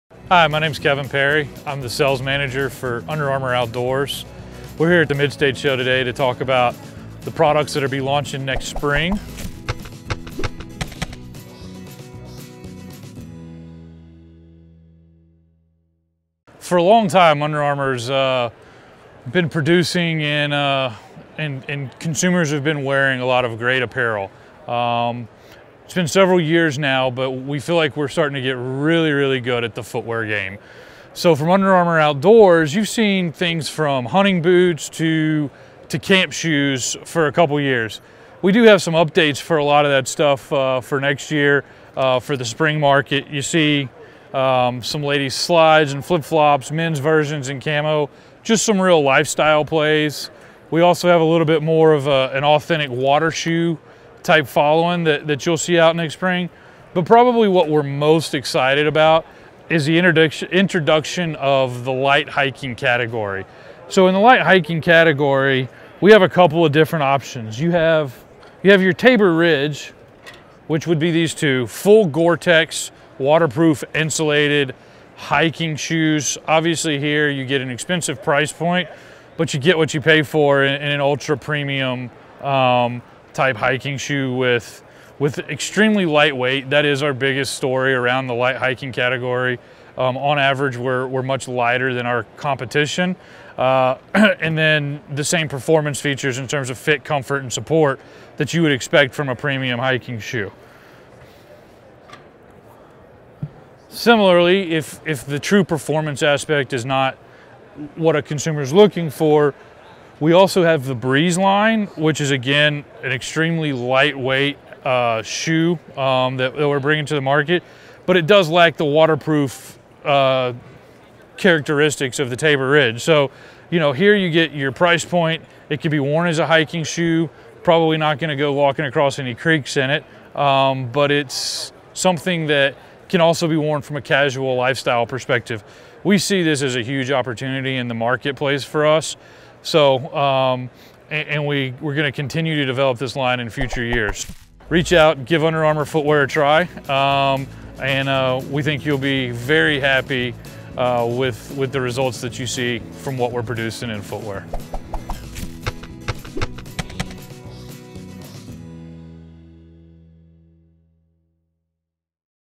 Under Armour Footwear
Under Armour provides a great selection of high quality footwear.  From athletic shoes to hunting boots, you can shop for a wide variety of Under Armour Footwear at C-A-L Ranch Stores.